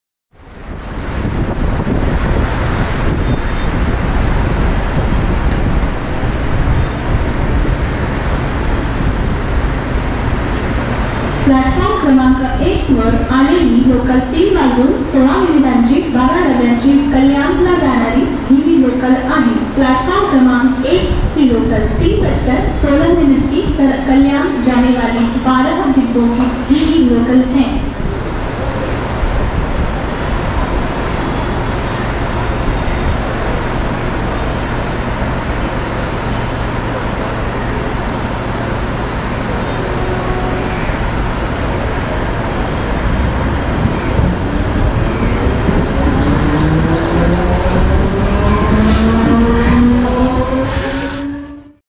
Kalyan Local Train Arrival & Announcement | Indian Railways 2022
My Dear Friends Watch the Kalyan Local Train arriving at Mumbra Railway Station. Indian Railways @MyTrainsINDIA